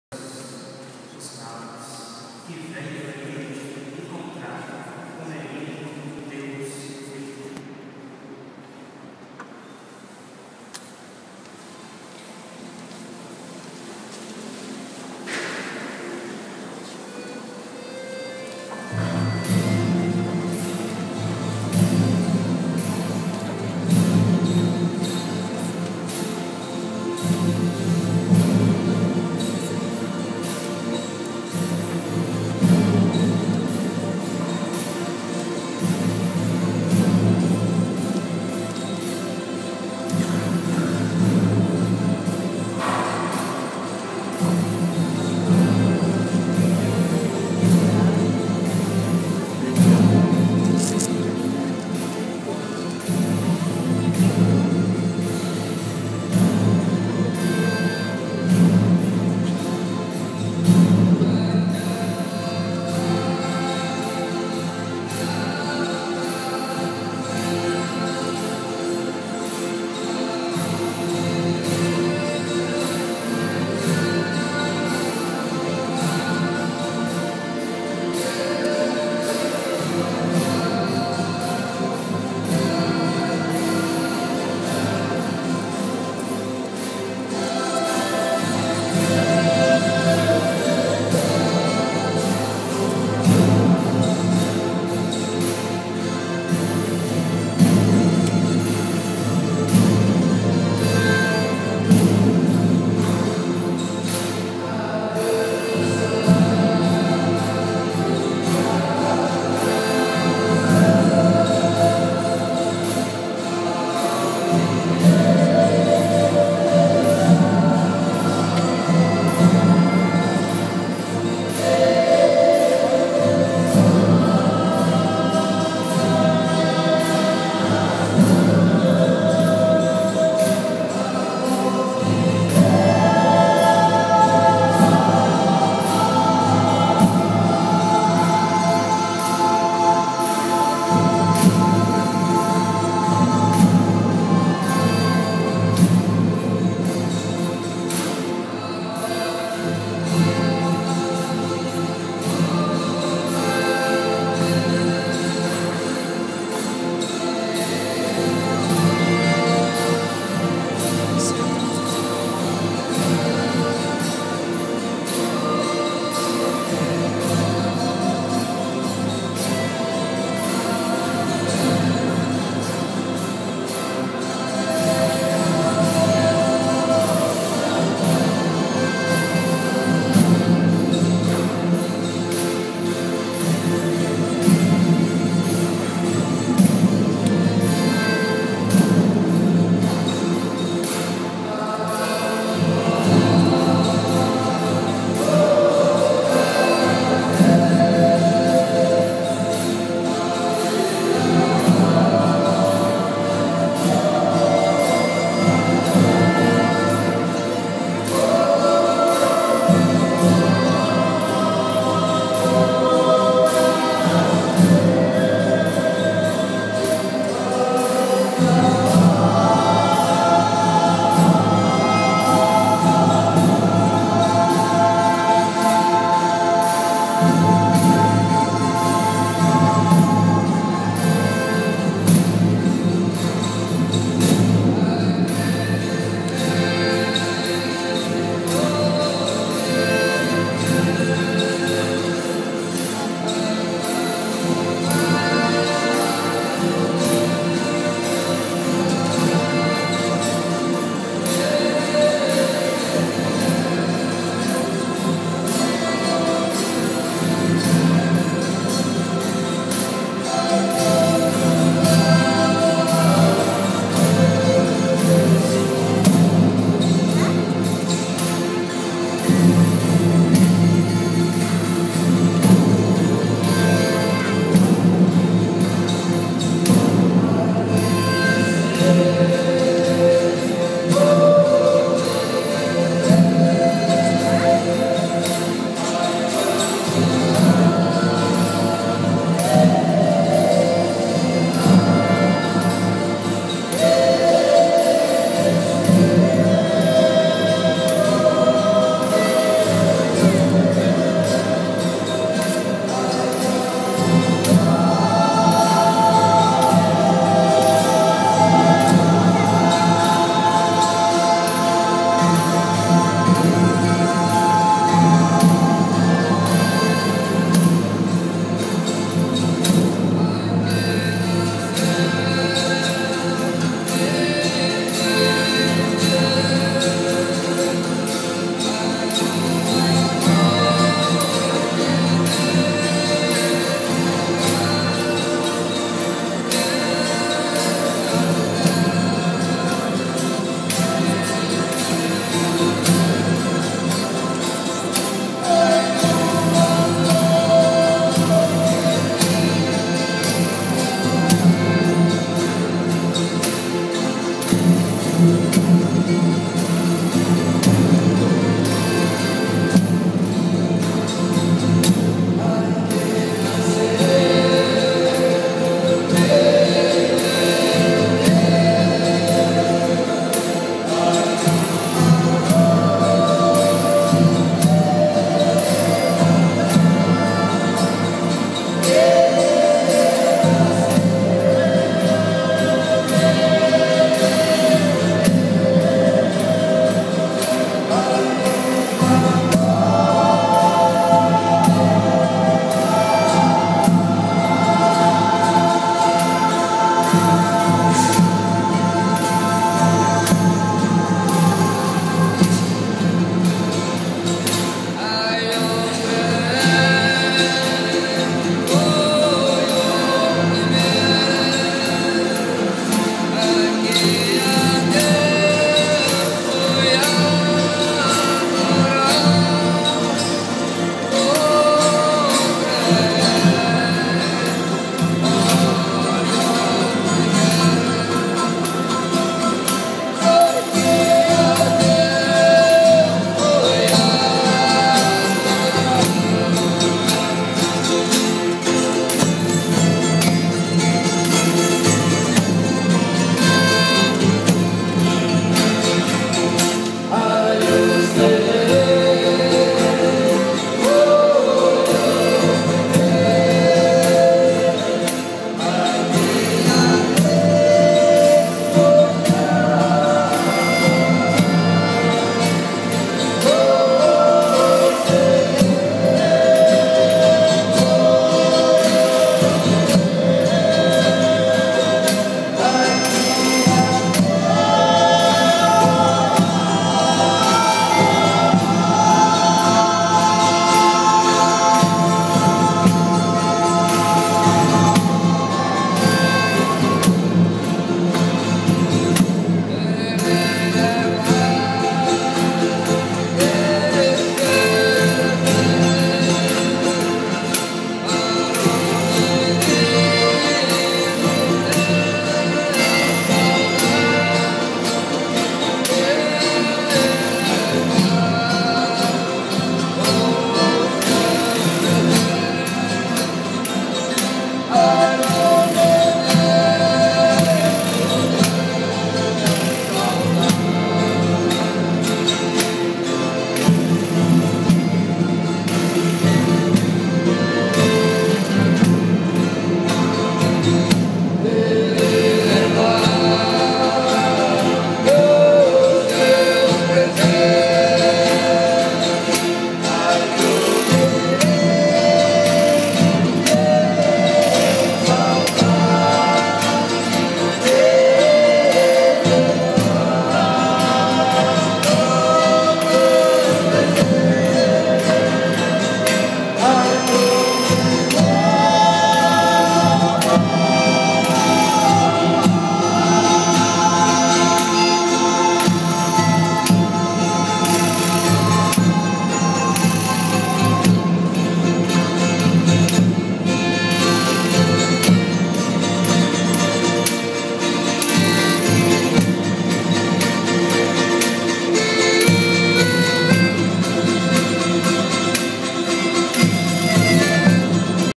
Companhia de Reis dos Ferroviários
Companhia de Reis dos Ferroviários, de Uberaba, MG, na Paróquia de São Domingos, dia 05 de janeiro de 2013, na missa da Epifania.